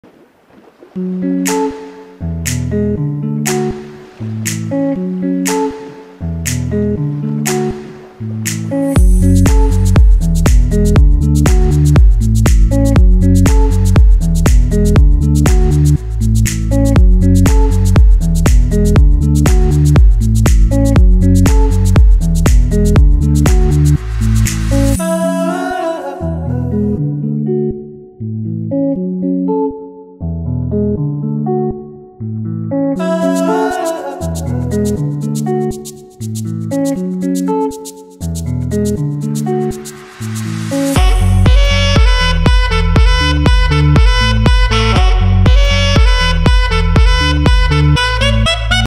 Homer, Alaska restaurants, food, and theater. If you haven’t visited, add it to the list!
Just a quick tour of some of the amazing restaurants in Homer, Alaska. This is no where near all of them and almost everyone is worth eating at. If you make your way to Homer make sure not to bring any food with you!

Also say a movie in the Homer theater, which was a very neat little theater.